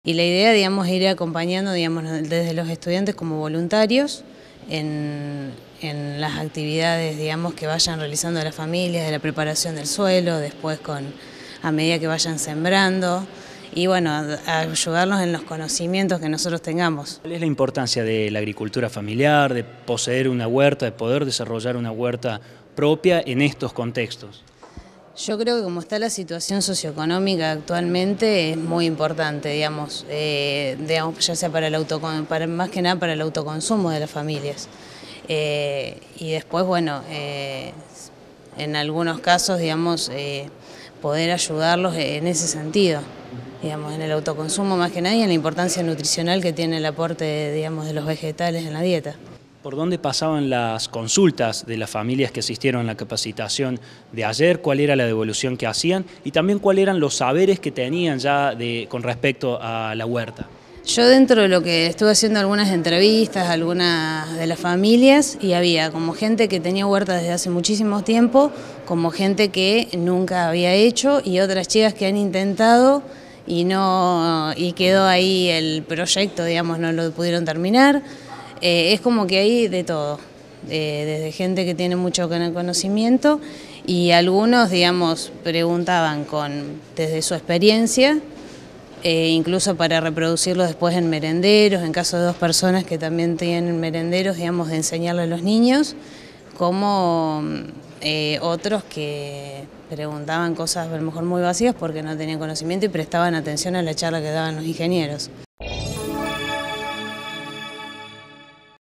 HUERTAS FAMILIARES Y COMUNITARIAS - GEORGINA MANDARADONI
LA MESA INTERINSTITUCIONAL CAPACITÓ A 30 FAMILIAS EN LA ESCUELA QUECHALÉN
Facebook.com/uniriotv
Twitter.com/uniriotv